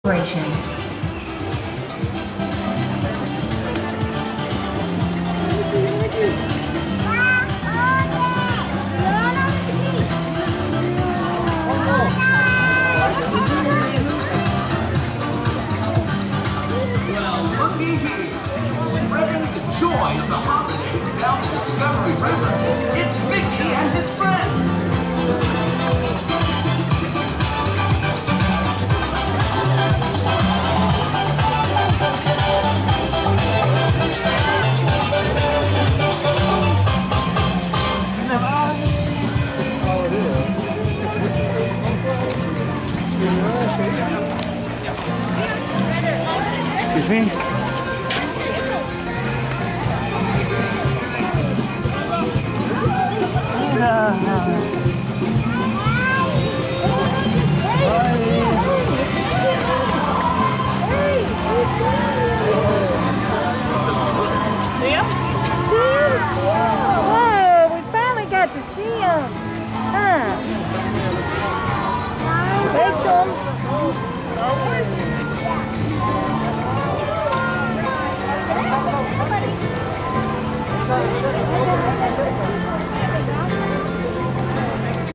Mickey and Friends Festive Flotilla Cavalcade Animal Kingdom Disney 2020
Mickey and Friends Festive Flotilla Cavalcade 2020

Marvel at Mickey and his pals as they head downriver on a holiday safari in a Festive Flotilla filled with Christmastime fun.

Walt Disney World Animal Kingdom